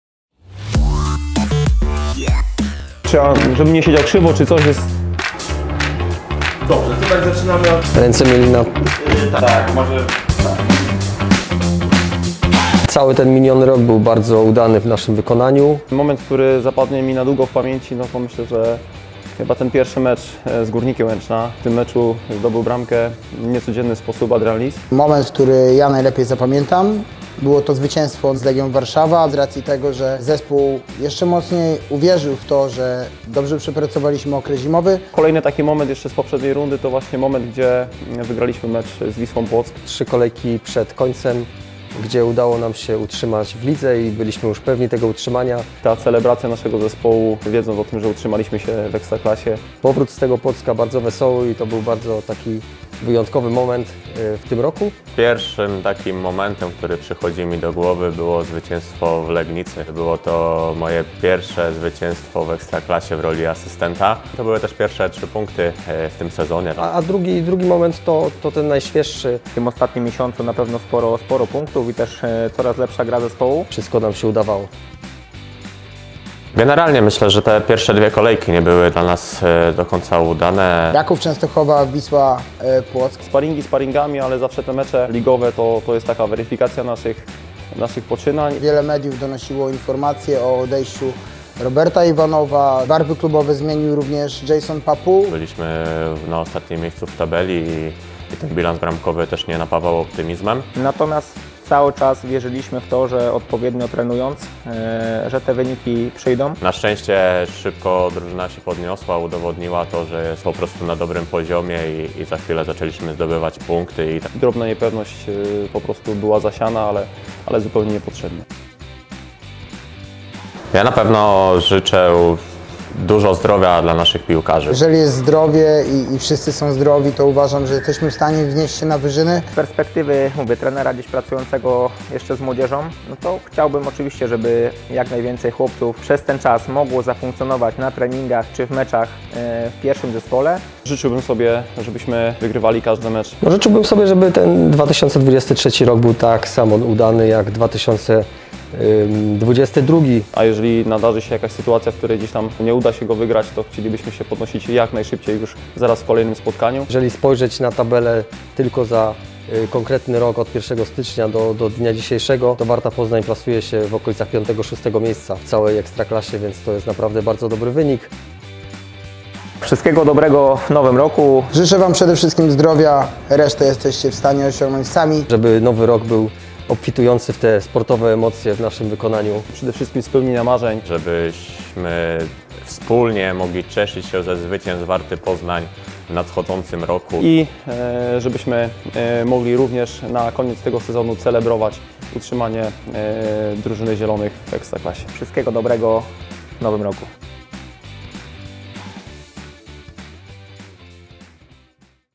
OBY KOLEJNY BYŁ TAKI JAK TEN. Sztab Warty Poznań podsumowuje rok 2022
Pierwsze od 85 lat zwycięstwo nad Legią Warszawa, kolejne utrzymanie w Ekstraklasie, tytuł lidera drużyn na wyjeździe. Koniec roku to również czas podsumowań. Zobacz, jak ten rok zapamiętali trenerzy ze sztabu pierwszego zespołu Warty Poznań.

🎥 Realizacja: Piotrek Przyborowski, Klaudia Berda

📸 Zdjęcia użyte w wideo:
Klaudia Berda / Warta Poznań